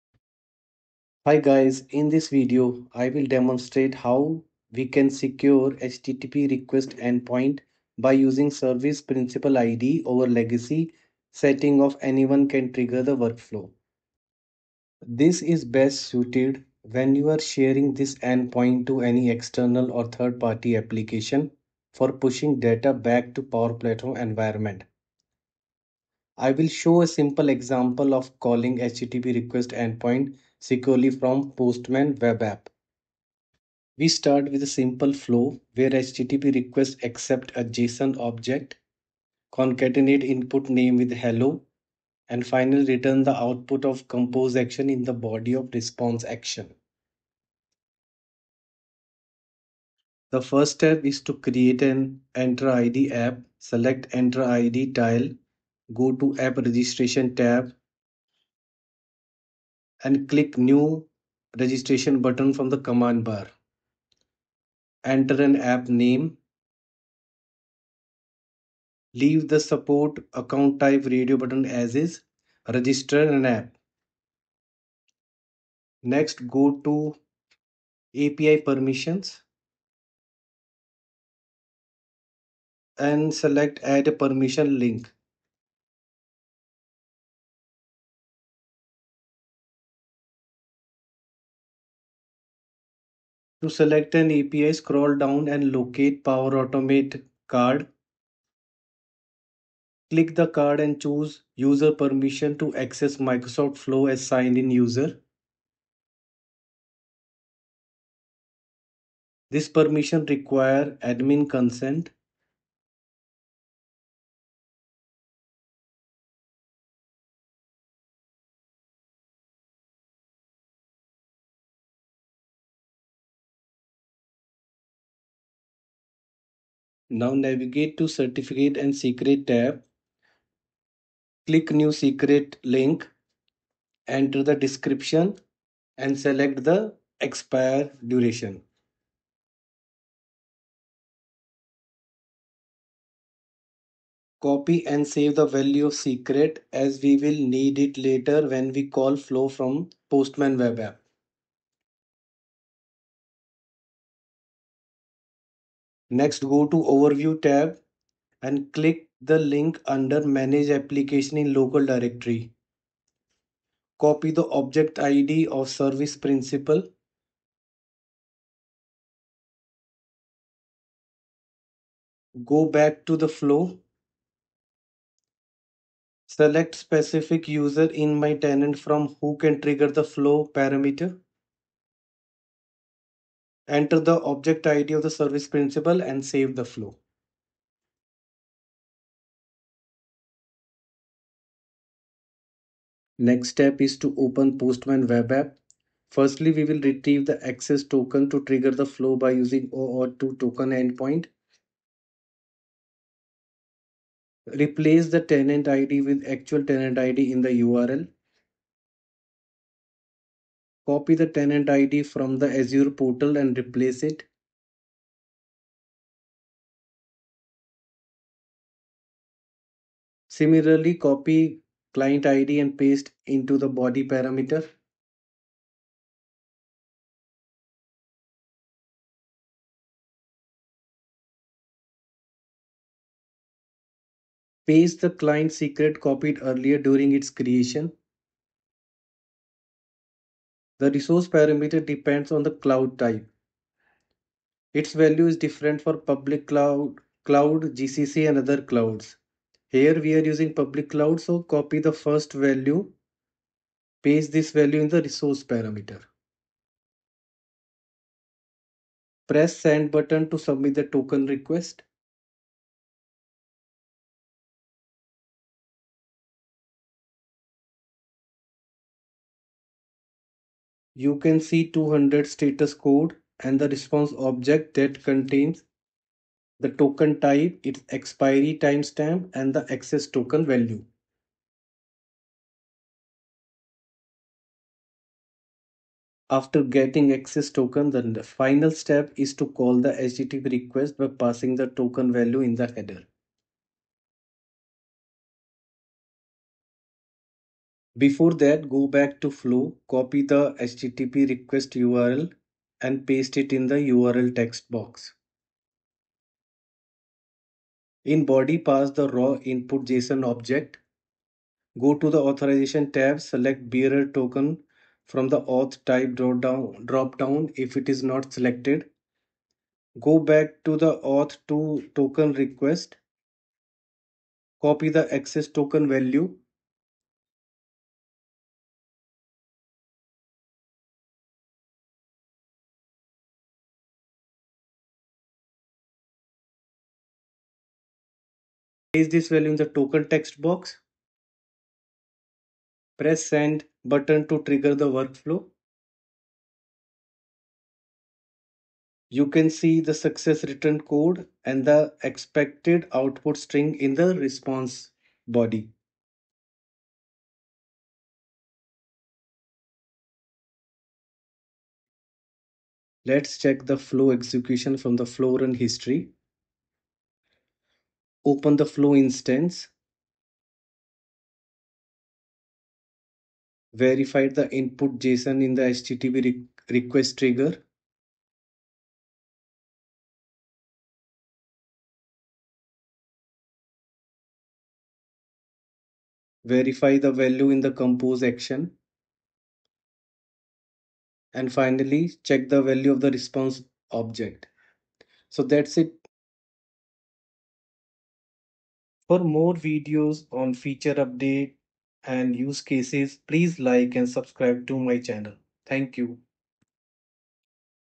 Secure HTTP Request EndPoint In Power Automate
Need help with Power Automate?
I offer expert consulting at $50/hour with a minimum 10-hour booking.

Video Overview:
Learn to set up a secure http request end point in power platform to exchange data with external or third party applications or systems.

It is a common scenario to exchange data between applications. In Power Platform, this is done through “HTTP” request trigger that supports exchange of data with external or 3rd party applications but the request end point should be secured to prevent breach.

In this video, I am going to demonstrate the 2-step process to invoke HTTP request endpoint from postman (external party). You can export the postman collection and share with external or 3rd party vendors to push data to this endpoint from their system.

This video covers the detailed steps:

▶️ Set up MicrosoftEntra ID app with appropriate permissions
▶️ Configure HTTP request endpoint
▶️ Call HTTP request endpoint from postman webapp (external party)